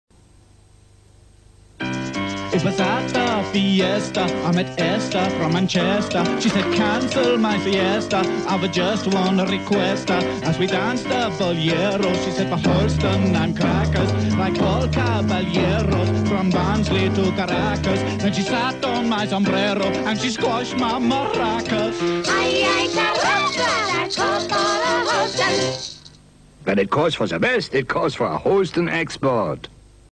Holsten Export advert - 1982
A jolly Mexican themed ad for the German lager. Not sure why, but hey, it's an excuse for the actors to wear oversized sombreros and fake moustaches as they sing about "Esther from Manchester" in the same sort of way that might get the presenters of Top Gear in lots or trouble.